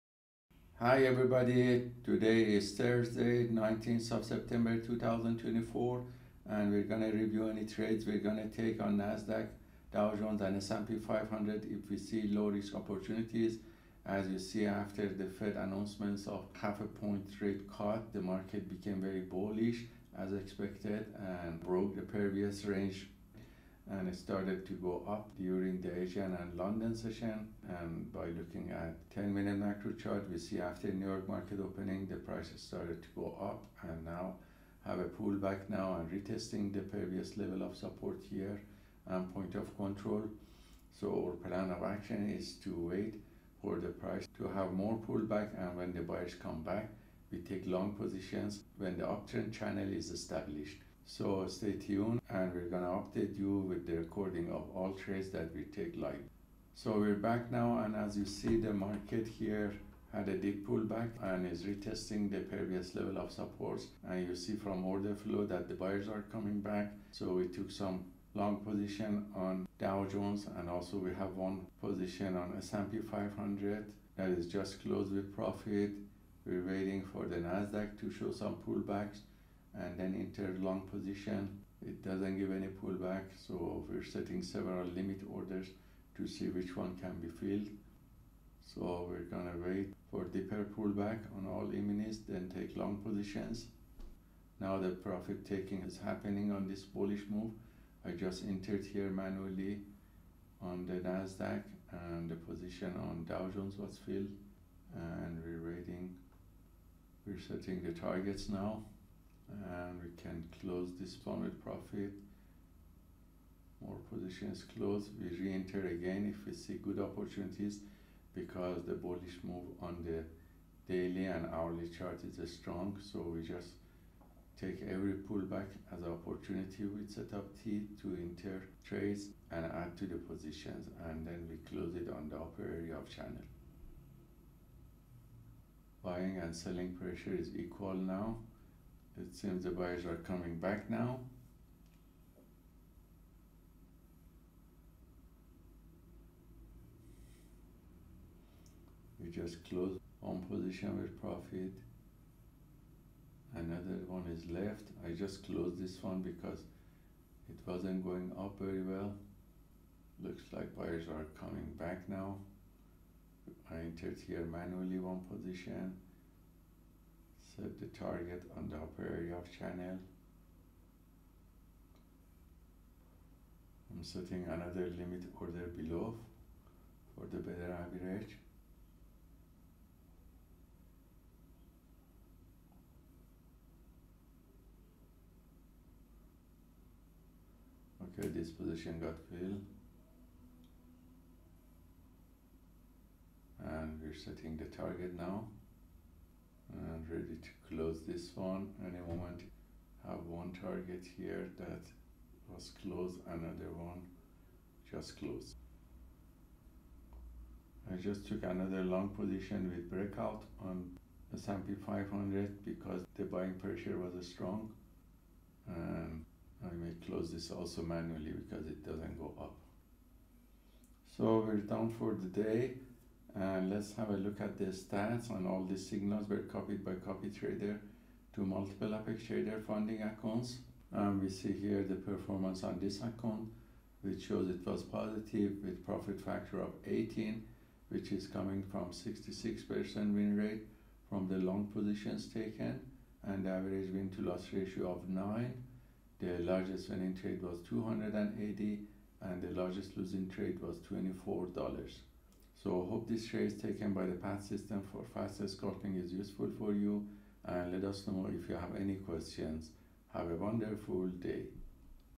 Price Action Algo Trading Live Trades/Journal: Nasdaq, Dow Jones, and S&P500 Futures - 19 Sep 2024
Price Action Algo Trading Live Trades/Journal: Nasdaq, Dow Jones, and S&P500 Futures - 19 September 2024